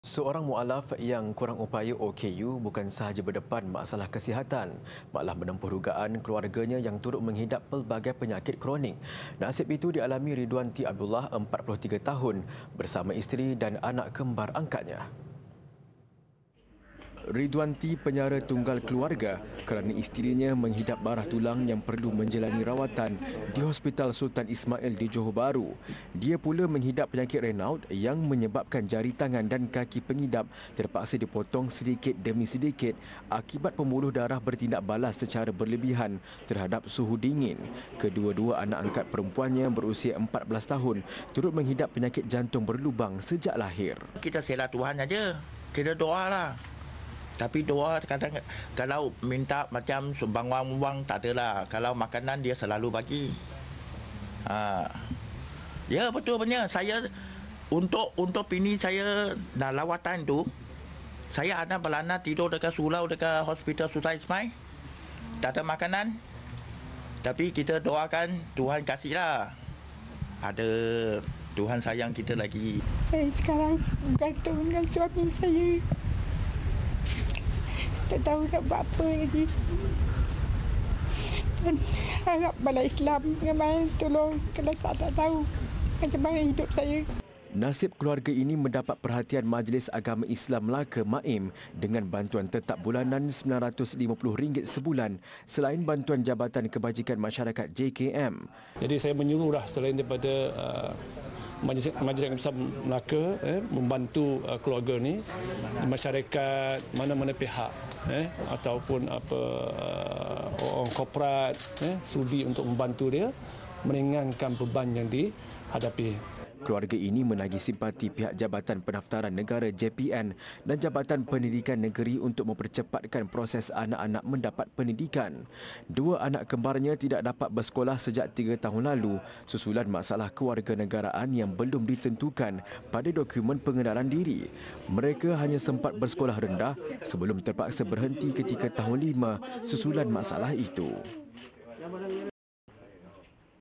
Dugaan Mualaf OKU, Isteri, Anak Hidapi Penyakit Kronik
Seorang mualaf yang kurang upaya (OKU) bukan sahaja berdepan masalah kesihatan, malah menempuh dugaan keluarganya yang turut menghidap pelbagai penyakit kronik.

Nasib itu dialami Ridhuan Tee Abdullah, 43 tahun, bersama isteri dan anak kembar angkatnya.